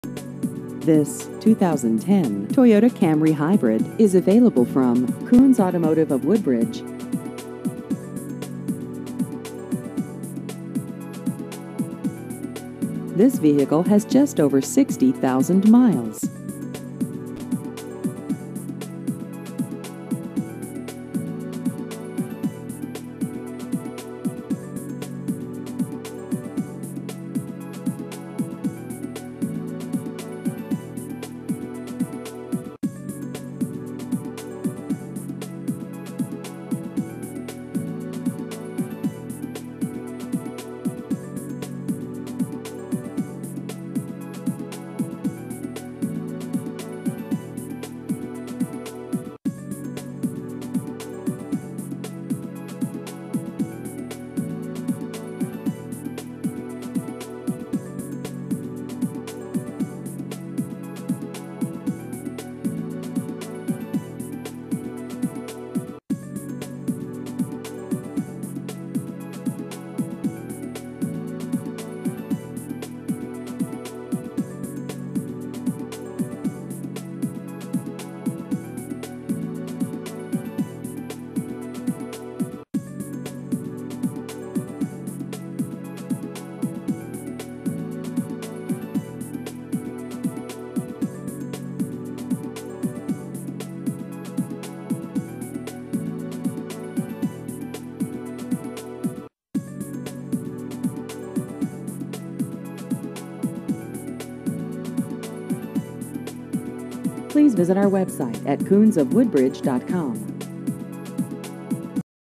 2010 Toyota Camry Hybrid Used Cars Woodbridge VA
This 2010 Toyota Camry Hybrid is available from Koons Automotive of Woodbridge.